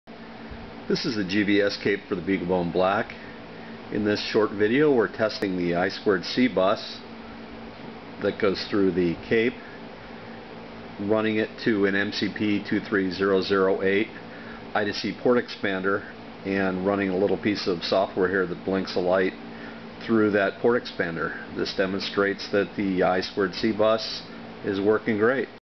BBB-GVS Cape with I2C working
Major breakthrough after several hours of work and a sleepless night, finally got the I2C pins tested. They worked. Here's the video to prove it. This is what we learned:
Hope this helps someone out there.